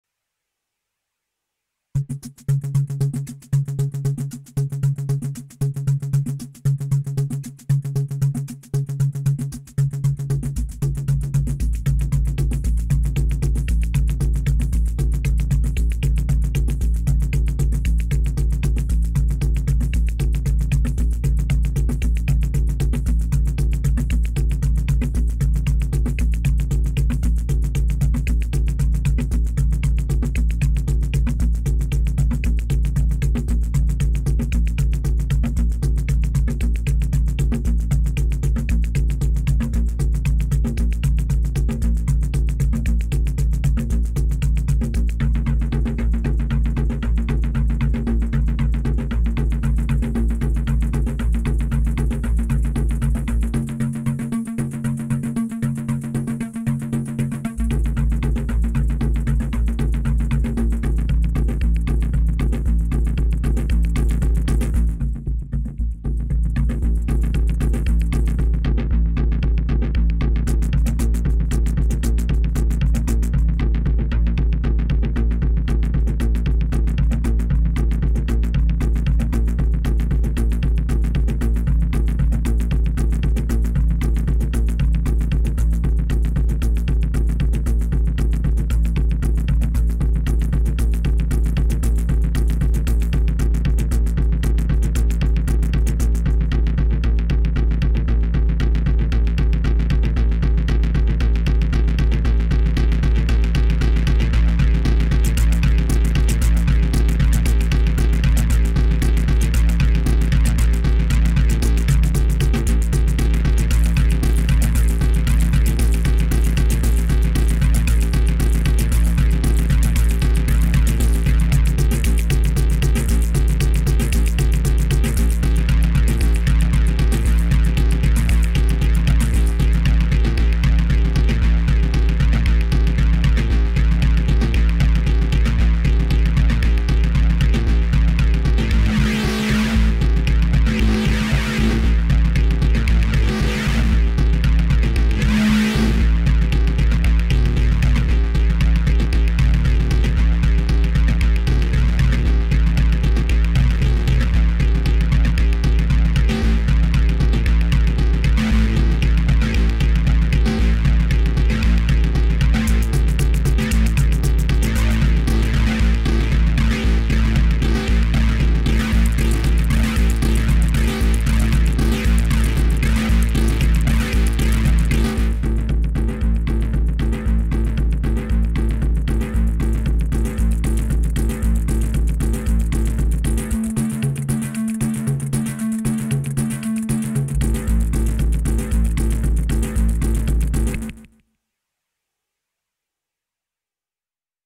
03 11 21 Scrotum Lab - Rhythm Ace FR-6 [02]
Trying different presets... The modified Rhythm Ace is now ready!